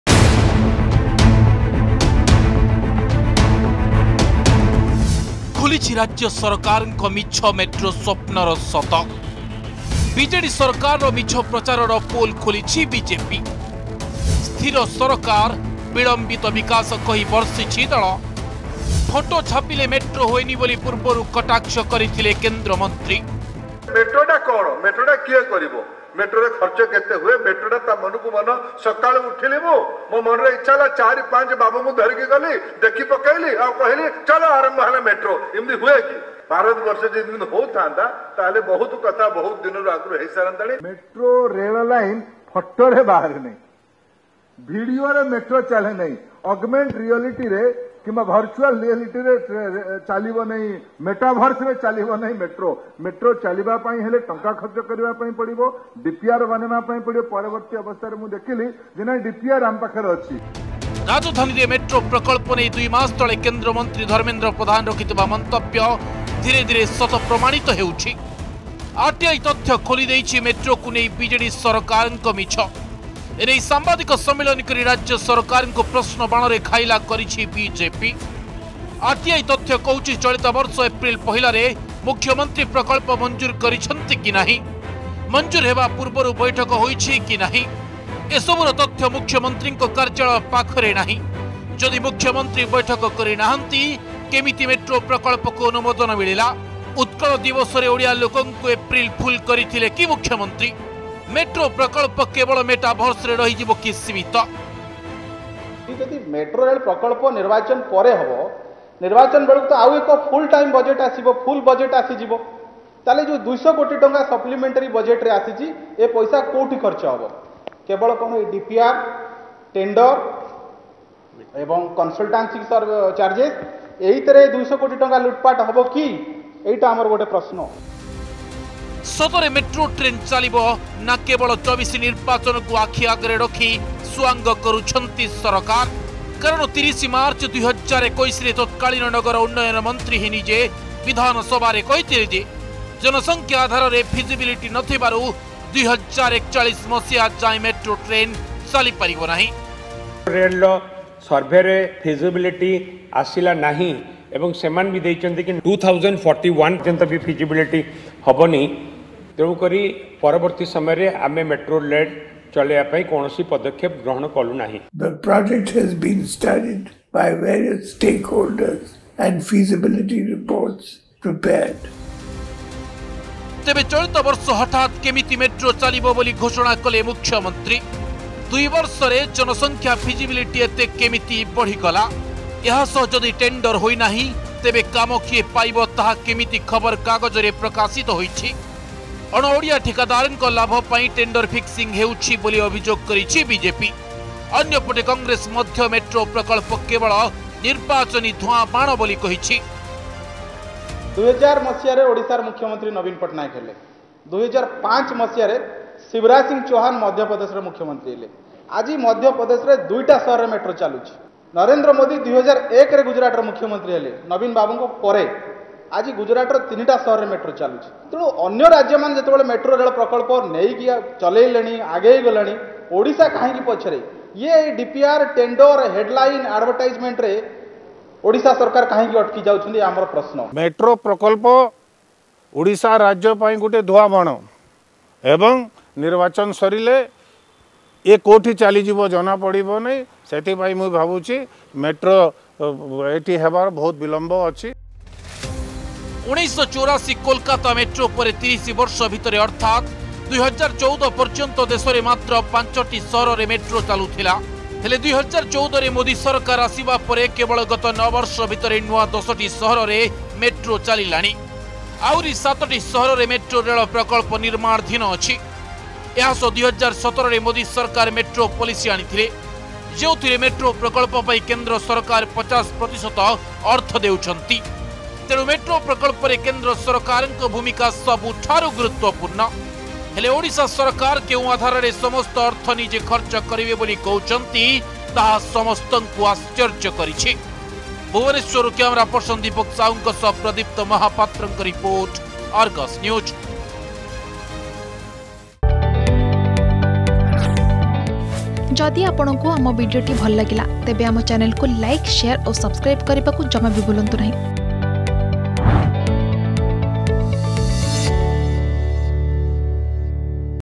ଖୋଲିଛି ରାଜ୍ୟ ସରକାରଙ୍କ ମିଛ ମେଟ୍ରୋ ସ୍ଵପ୍ନର ସତ || Metro Project || Odisha Govt
ଉତ୍କଳ ଦିବସରେ ଓଡ଼ିଆ ଲୋକଙ୍କୁ ଏପ୍ରିଲ ଫୁଲ କରିଥିଲେ କି ସରକାର ।  ଆରଟିଆଇ ତଥ୍ୟ ଖୋଲିଲା ରାଜ୍ୟ ସରକାରଙ୍କ ମିଛ ମେଟ୍ରୋ ସ୍ଵପ୍ନର ପୋଲ୍‌ । ବିନା ବୈଠକରେ ପ୍ରକଳ୍ପକୁ କେମିତି ଅନୁମୋଦନ ମିଳିଲା ବୋଲି ପ୍ରଶ୍ନ ଉଠାଇଛି ବିଜେପି ।